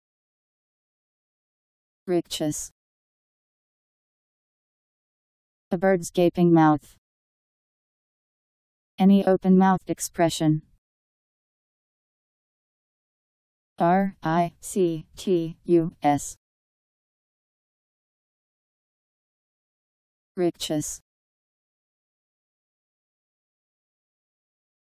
Rictus Meaning
Video shows what rictus means. A bird's gaping mouth. Any open-mouthed expression.  Rictus Meaning. How to pronounce, definition audio dictionary. How to say rictus. Powered by MaryTTS, Wiktionary